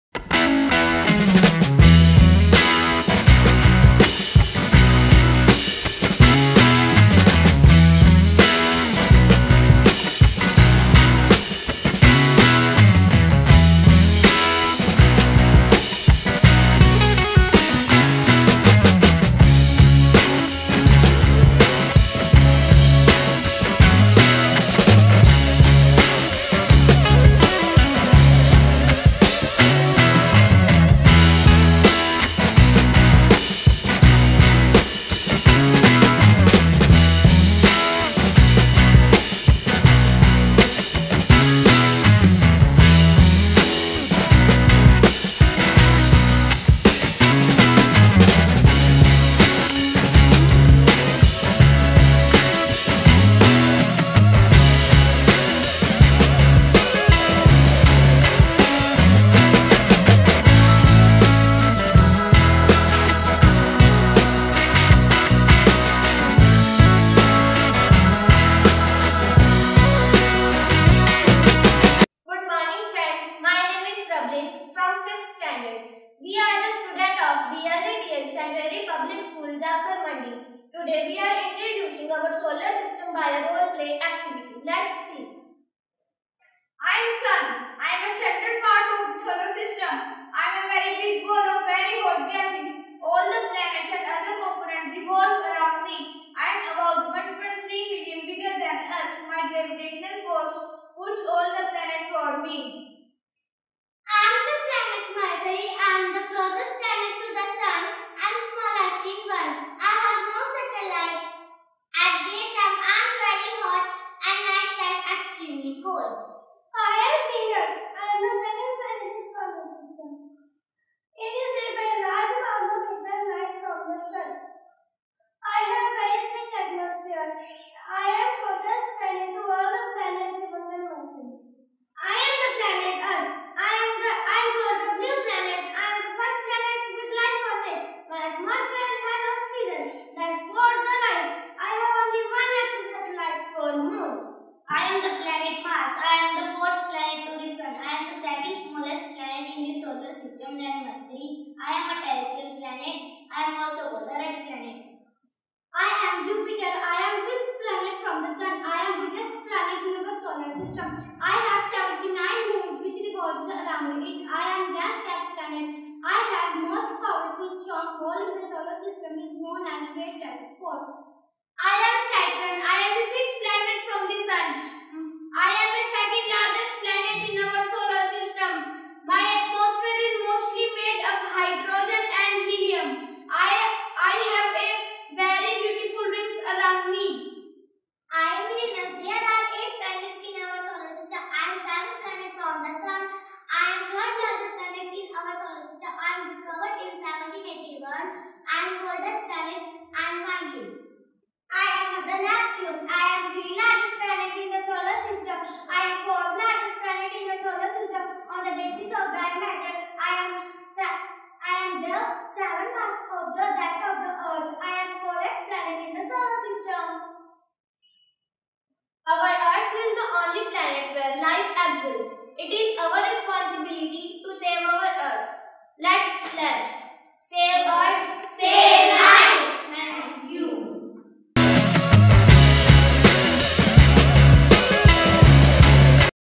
Role play Activity on Solar system
In this video you will watch our 5th standard students are performing a Role play activity on Solar system @ BLJ DAV Cent. Public School, Jakhal. Students are learning about our planets with the help of this activity. Our students are enjoying their study by performing interesting activities. This is a science activity and Ms. Pinky Bedi was the incharge of this activity.